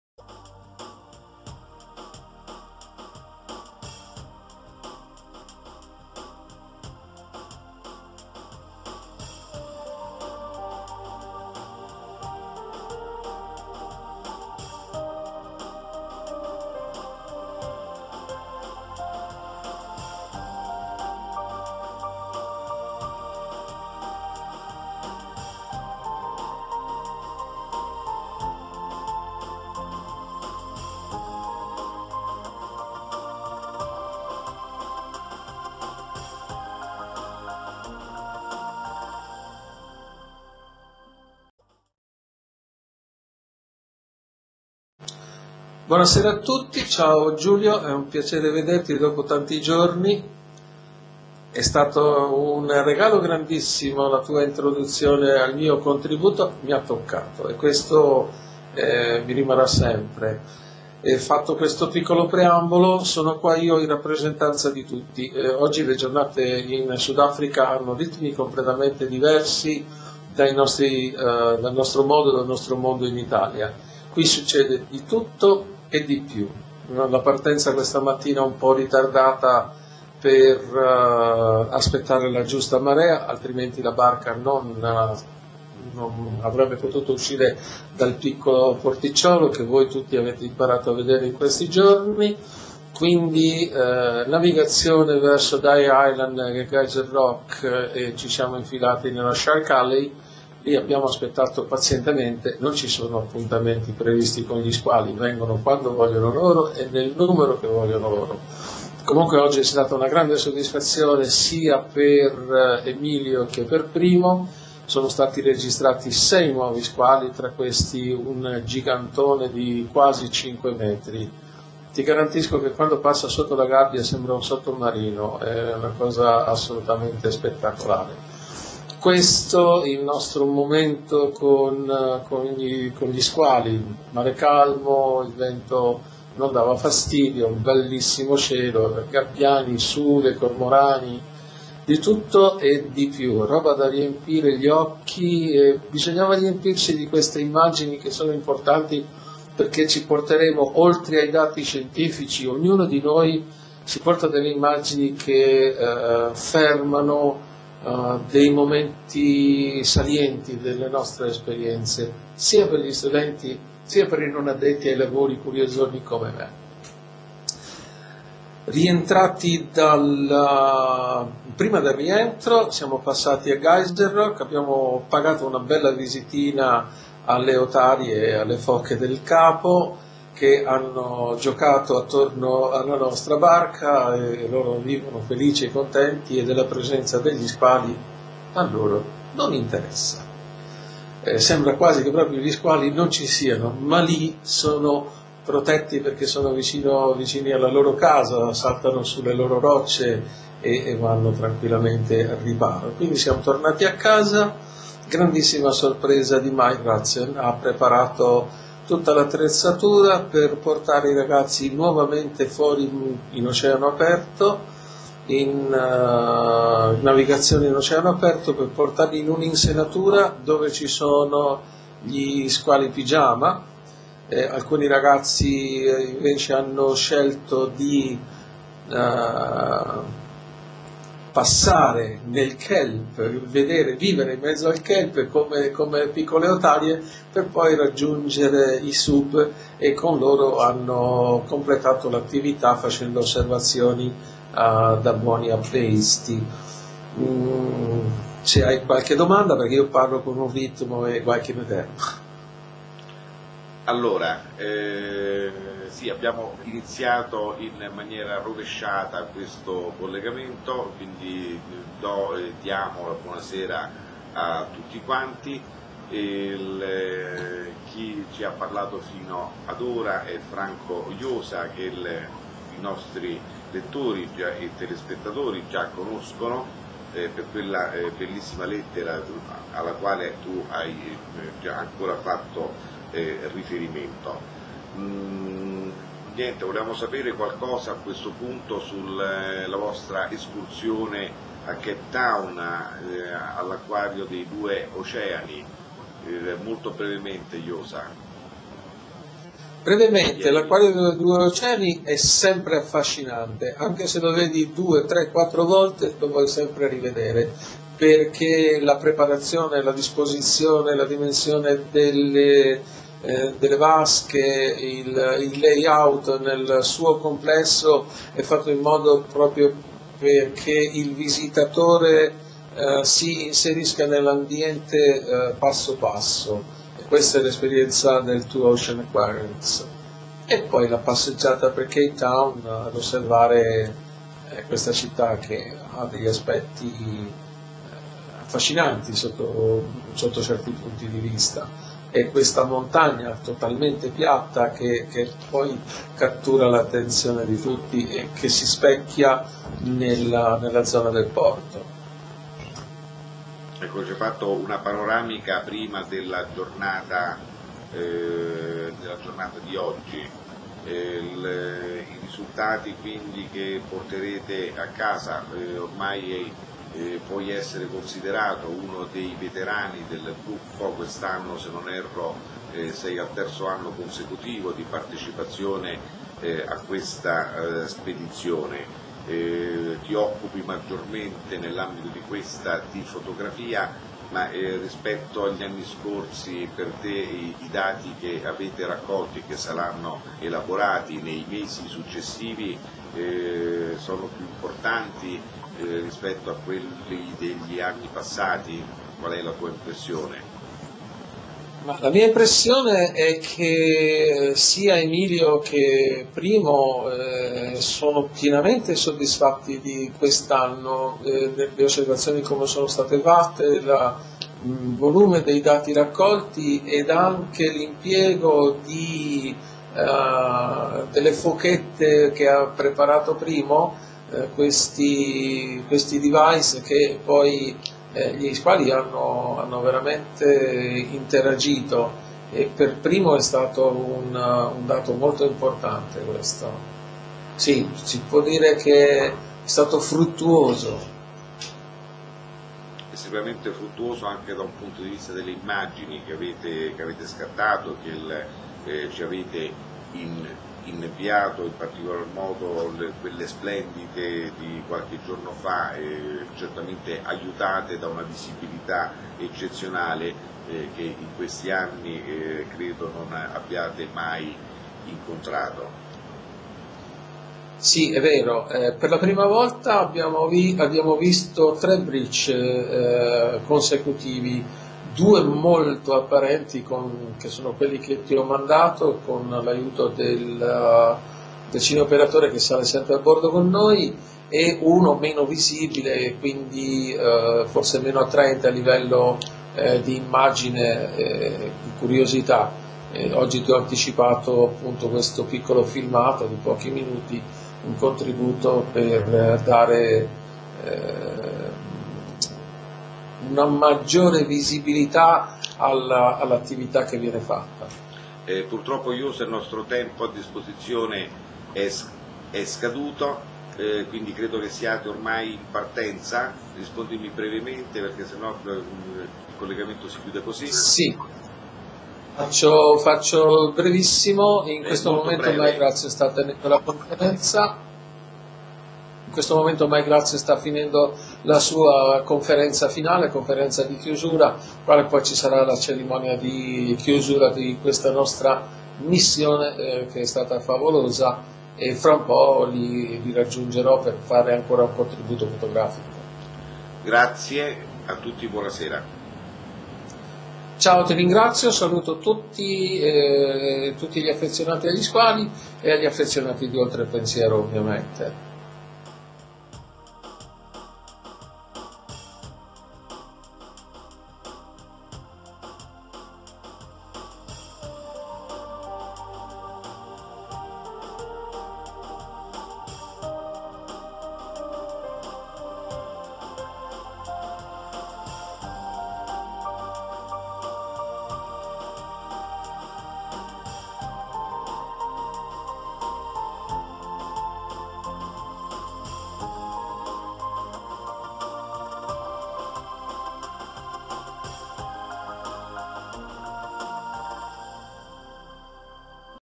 "DIARIO DI BORDO" 2010 - 23/24 Aprile VII SPEDIZIONE SQUALO BIANCO IN SUDAFRICA
Nel video, in collegamento da Gansbaay,
Franco Iosa
Subacqueo - Fotografo
VII Spedizione Scientifica in SudAfrica
"Sulle Orme del Grande Squalo Bianco"

Unità di Studio e Ricerca sugli Squali Bianchi
Aquarium Mondo Marino

Videopensiero e la WebTv di